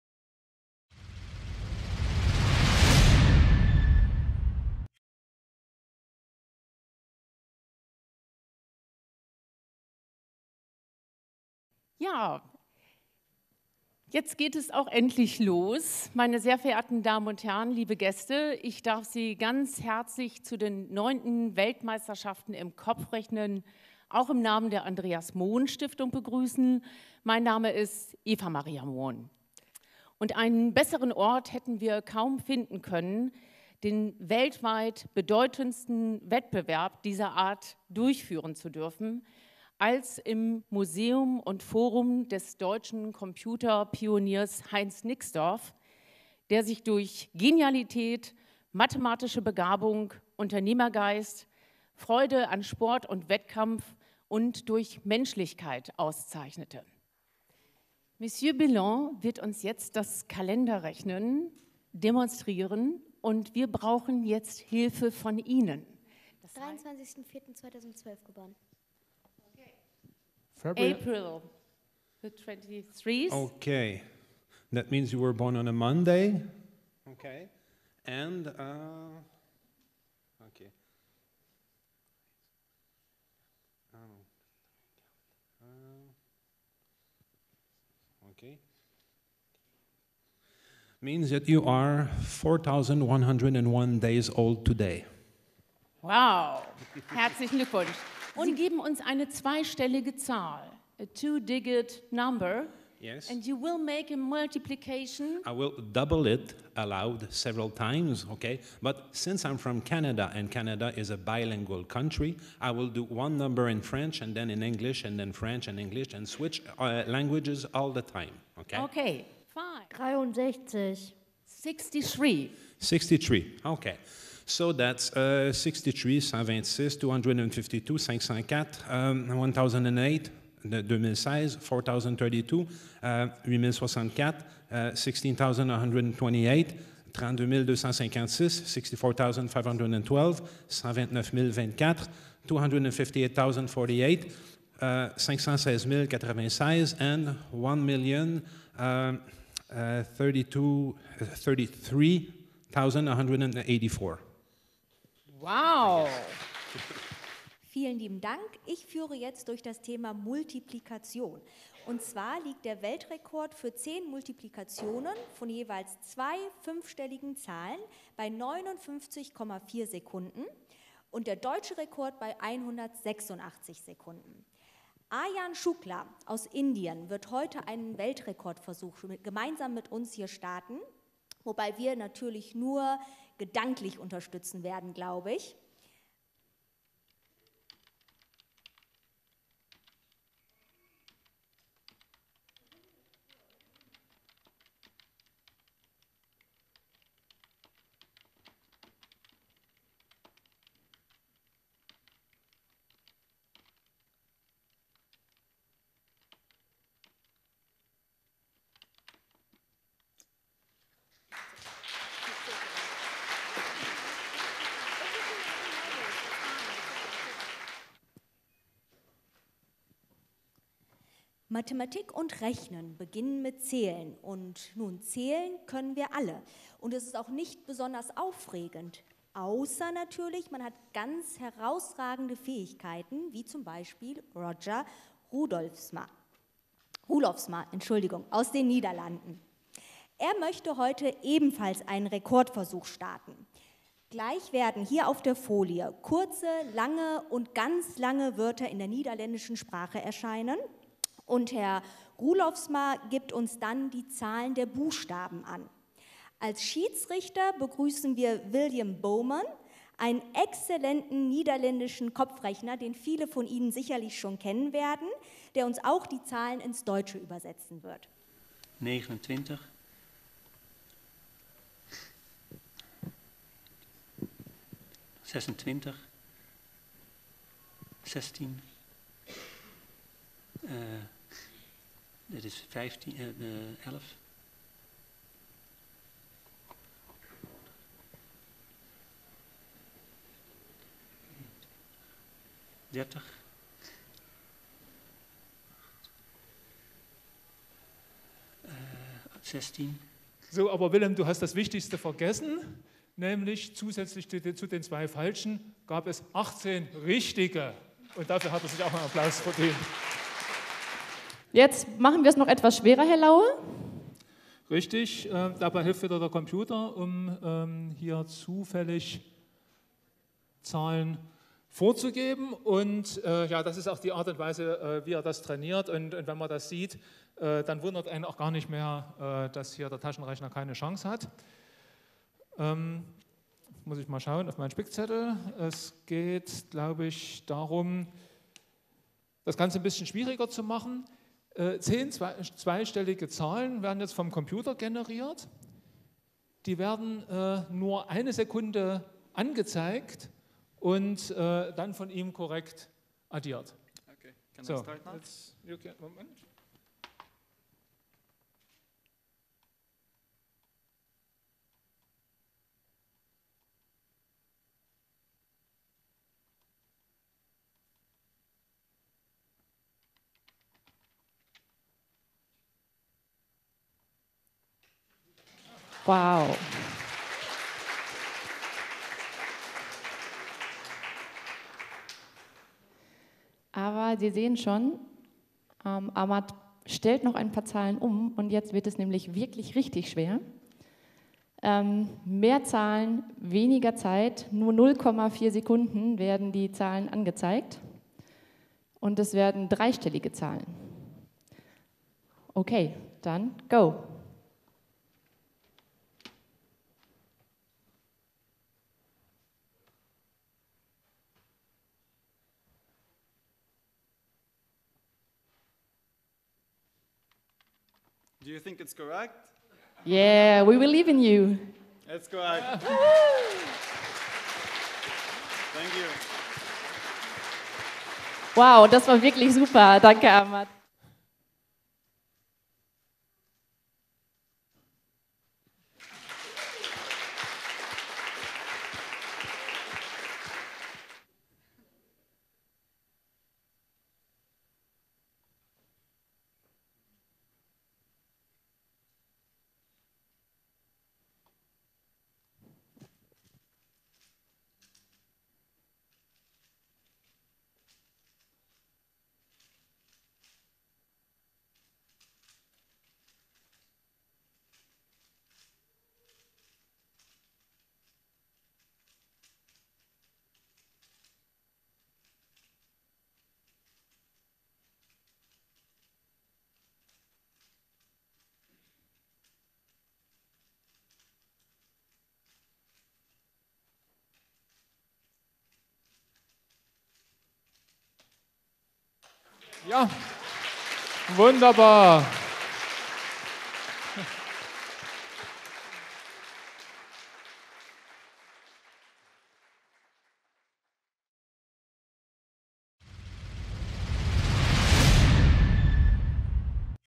9. Weltmeisterschaften im Kopfrechnen - Showveranstaltung
Im Heinz Nixdorf MuseumsForum trafen sich vom 15. bis 17. Juli 2022 die weltbesten Rechenkünstler zur neunten Auflage der Weltmeisterschaften im Kopfrechnen.

In einer Showveranstaltung gaben die Hirnakrobaten bei einer öffentlichen Vorstellung eine Kostprobe ihres Könnens. Zu sehen sind das Ziehen der dritten Wurzel aus einer 18- bis 21-stelligen Zahl. Schneller als ein Taschenrechner ermittelt ein Teilnehmer das Additionsergebnis von zehn dreistelligen Zahlen. Außergewöhnliche Gedächtnisleistungen und das rasante Lösen des Zauberwürfels in zehn Sekunden werden ebenfalls demonstriert.